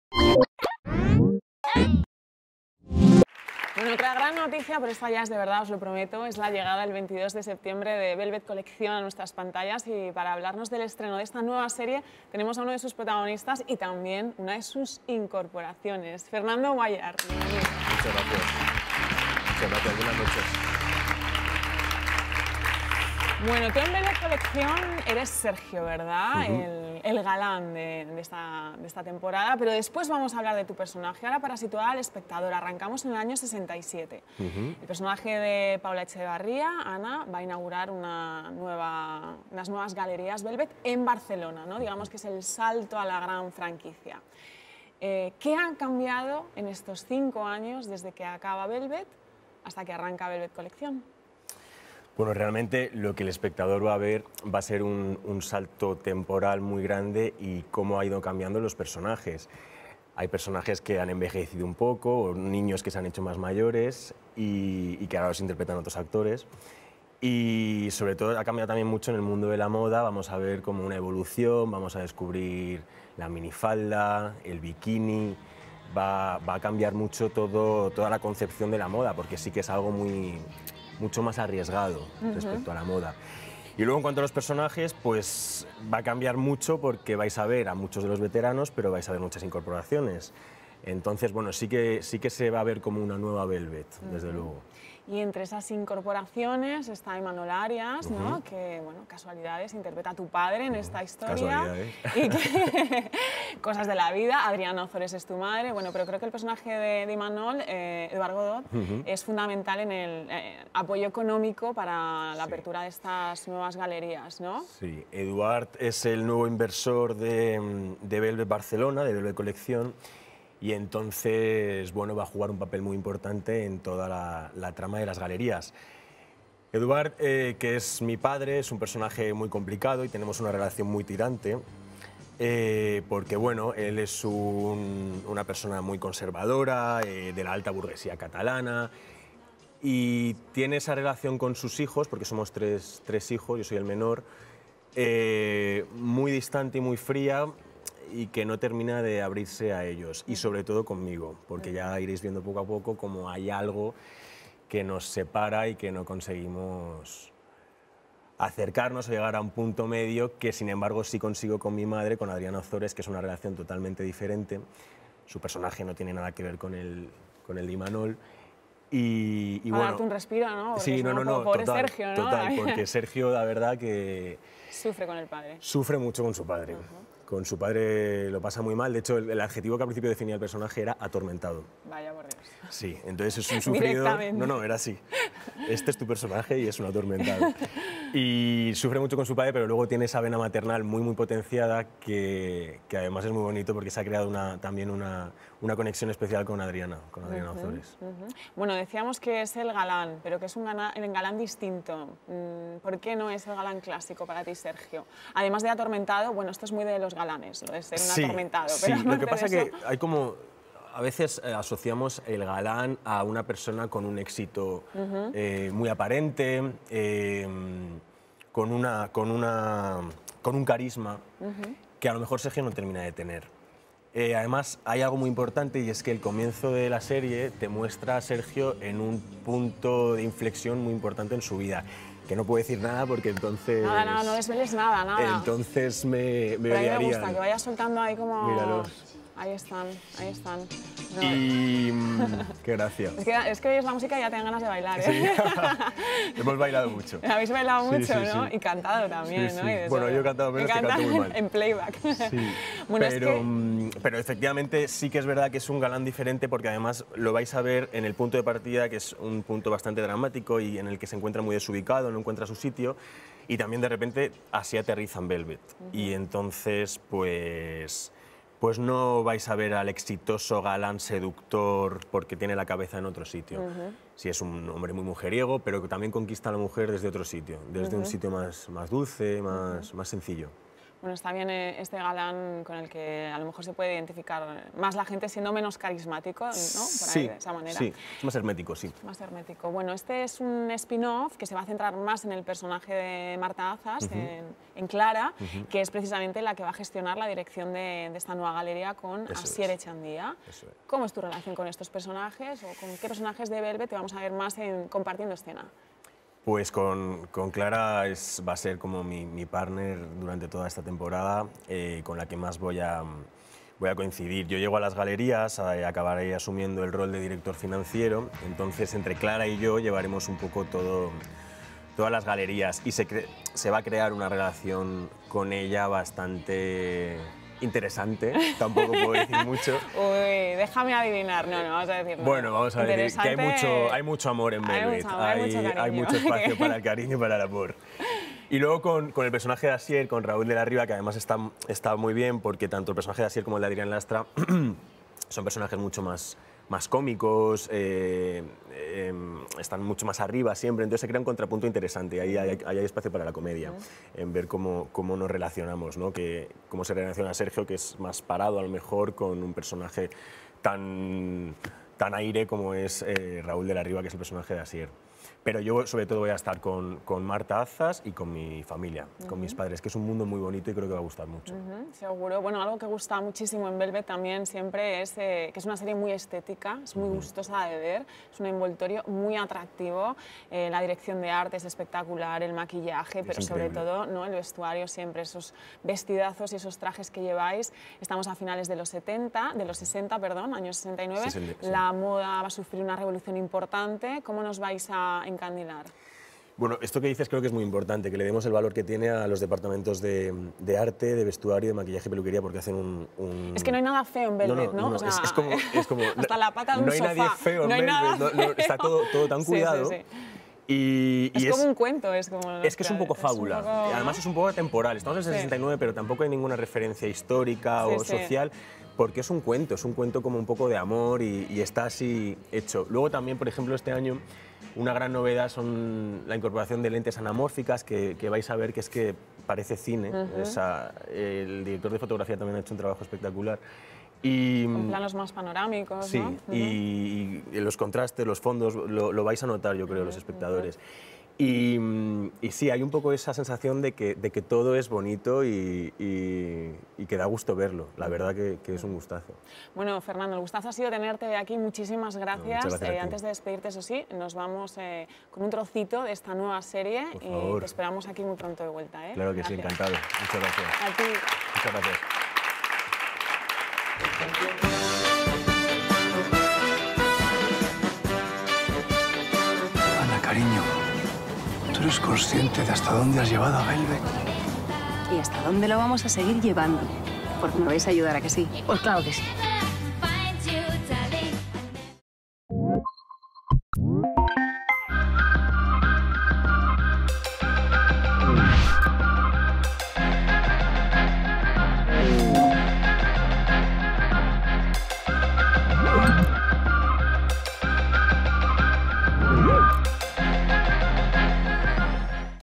Pool Fiction: Fernando Guallar nos presenta "Velvet Colección" | Movistar+
Cristina Teva entrevista a Fernando Guallar, una de las incorporaciones a "Velvet Colección".